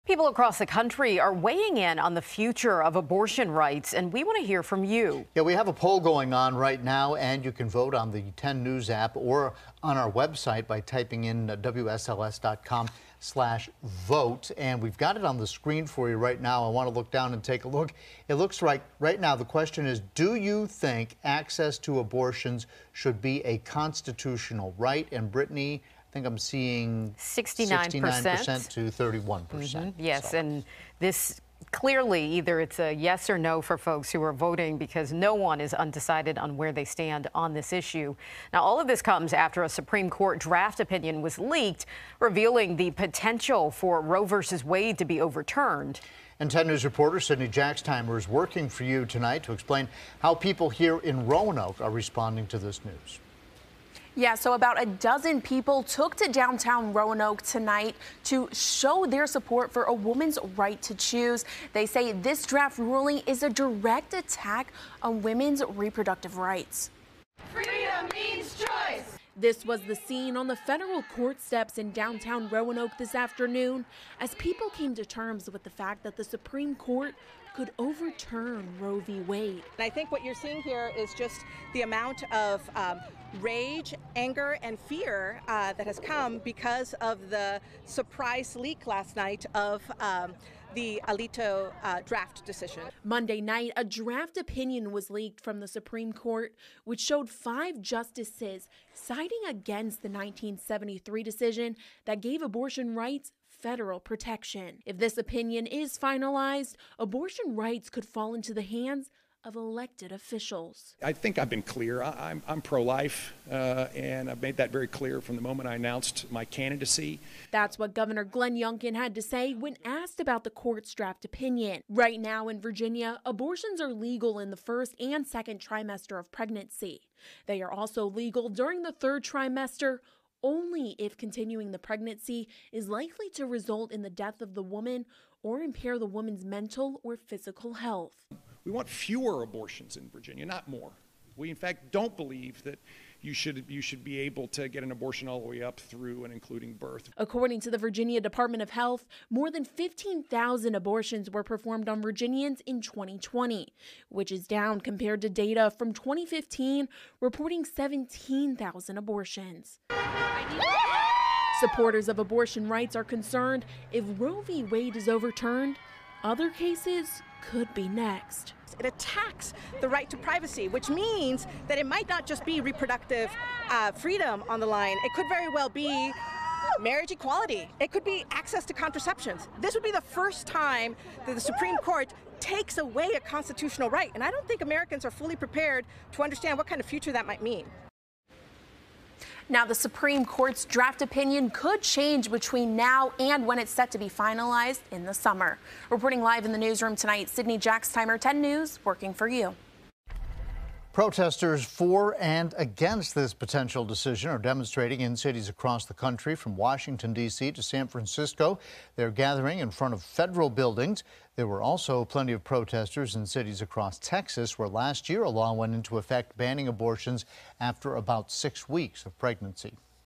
Protestors support abortion rights in downtown Roanoke
People across the country are weighing in on the future of abortion rights.